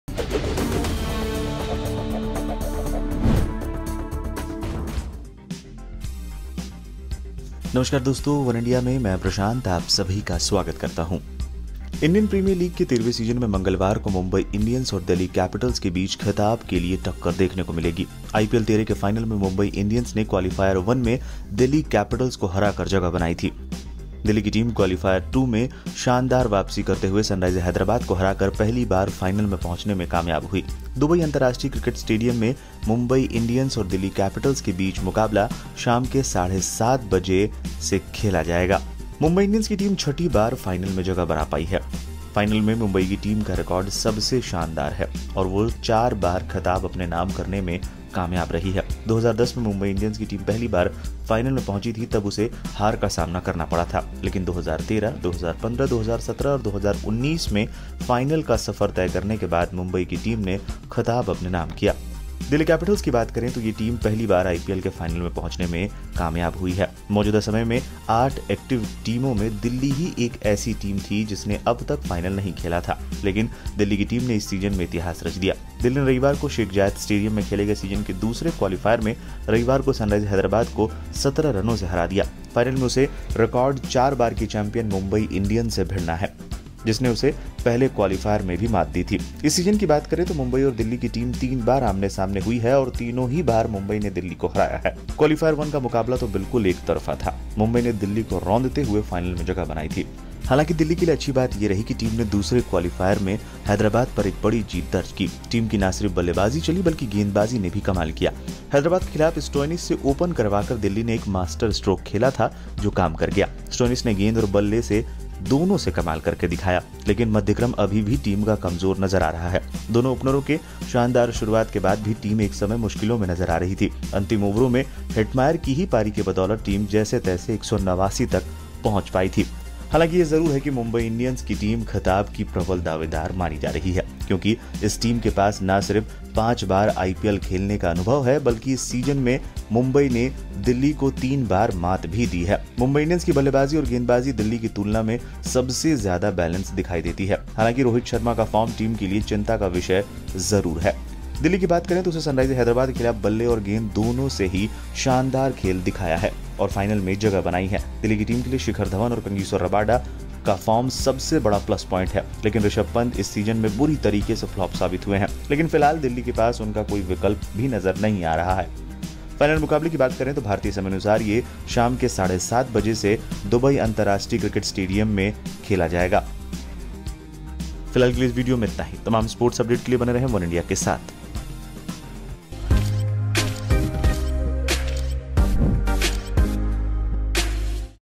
IPL 2020 Final MI vs DC: Mumbai look for fifth title, Delhi Capitals want 1st one | Oneindia Sports
Mumbai Indians will be thoroughly challenged by the young Mumbaikar Shreyas Iyer, who will lead a passionate but unpredictable Delhi Capitals in a of an Indian Premier League final here on Tuesday.After 52 days of the most closely-contested and widely-watched IPL played on foreign land in the backdrop of the raging Covid-19 pandemic, the BCCI can pat itself on the back for pulling off a near flawless tournament that gave people a chance to shift from prime-time negativity that dominated air space.

इंडियन प्रीमियर लीग के 13वें सीजन में मंगलवार को मुंबई इंडियंस और दिल्ली कैपिटल्स के बीच खिताब के लिए टक्कर देखने को मिलेगी. आईपीएल 13 के फाइनल में मुंबई इंडियंस ने क्वालिफायर वन में दिल्ली कैपिटल्स को हराकर जगह बनाई है. दिल्ली की टीम क्वालिफायर टू में शानदार वापसी करते हुए सनराइजर्स हैदराबाद को हराकर पहली बार फाइनल में पहुंचने में कामयाब हुई. दुबई अंतरराष्ट्रीय क्रिकेट स्टेडियम में मुंबई इंडियंस और दिल्ली कैपिटल्स के बीच शाम 7.30 बजे 13वें सीजन का फाइनल मैच खेला जाएगा.